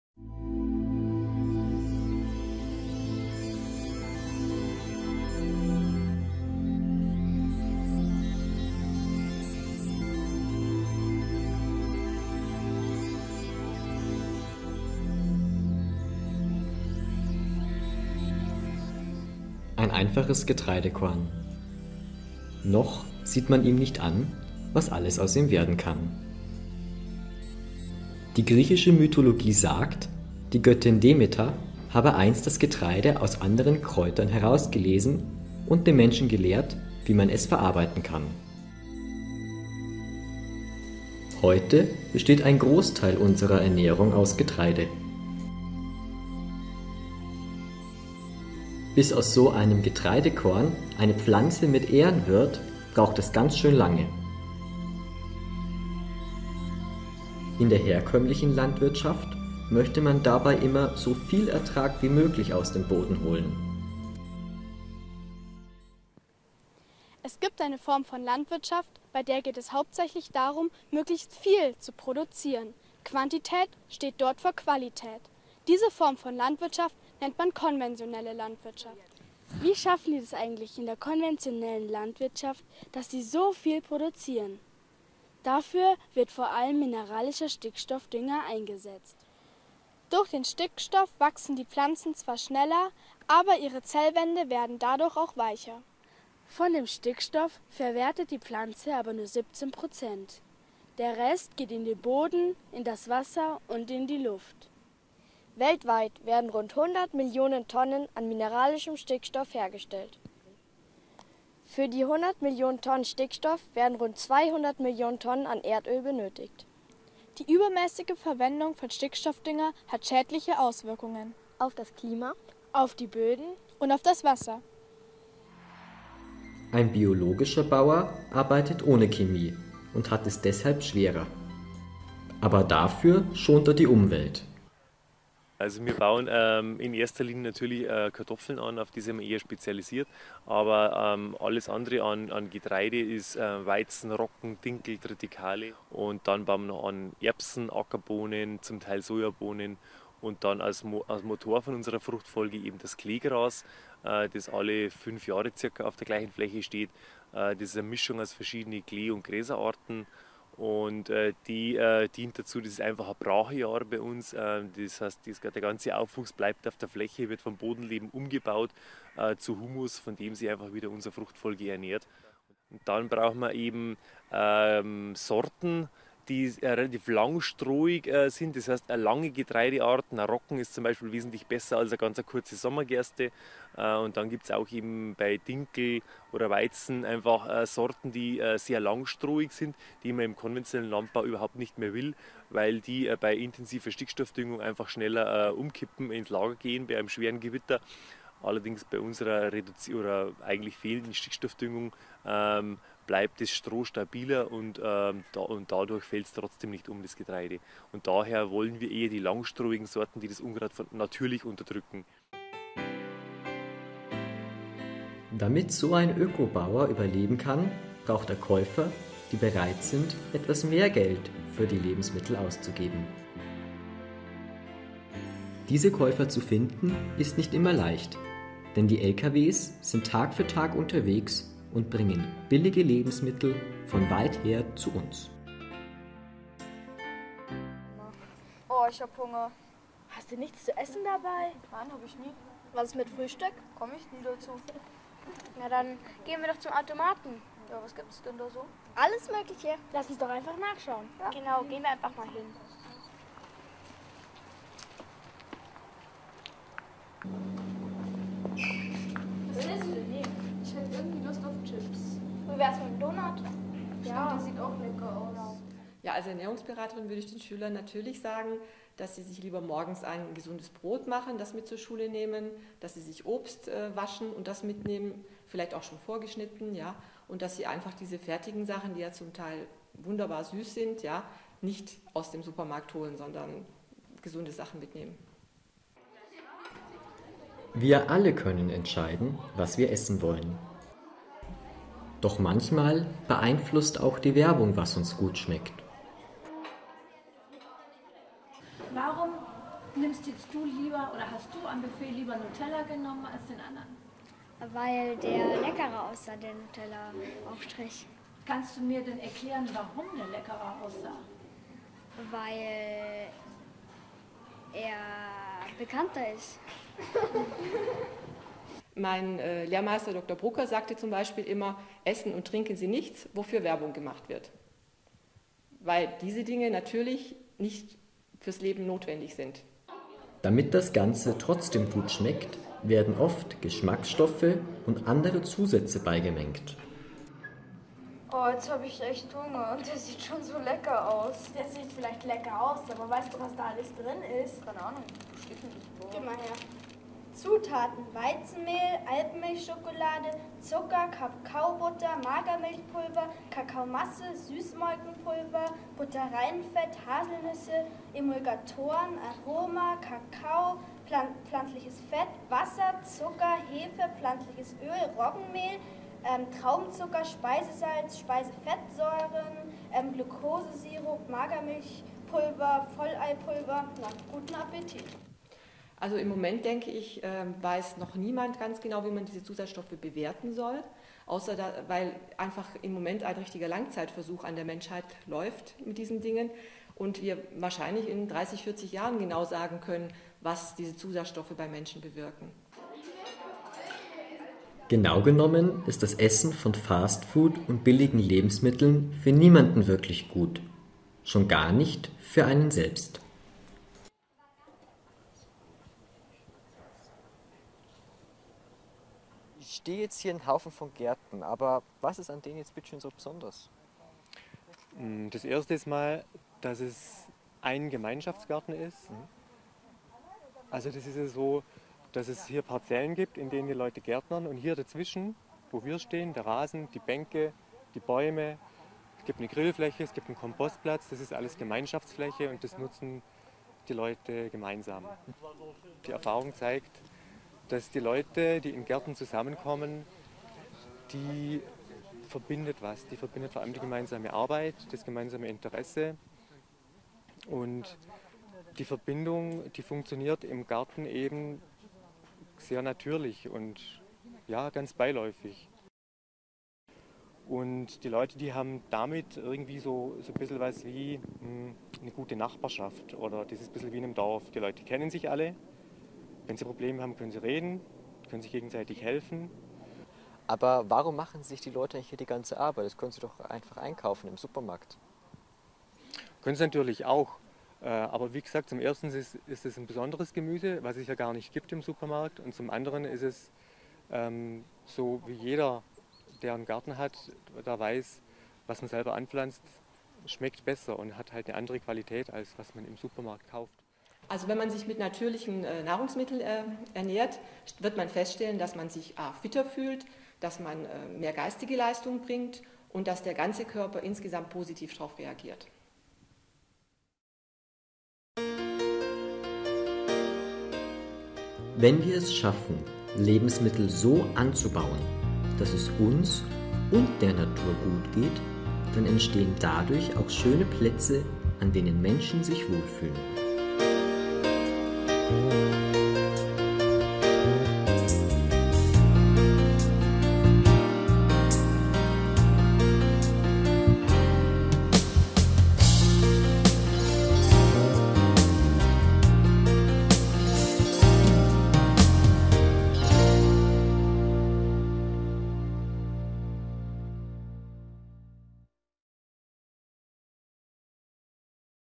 Klima und Ernährung
Die Umweltschule am Ernst-Mach-Gymnasium Haar hat sich im Jahr 2012 mit dem Thema "Klima und Ernährung" beschäftigt.

Mit Unterstützung der Gemeindewerke Haar ist daraus ein Projektfilm entstanden.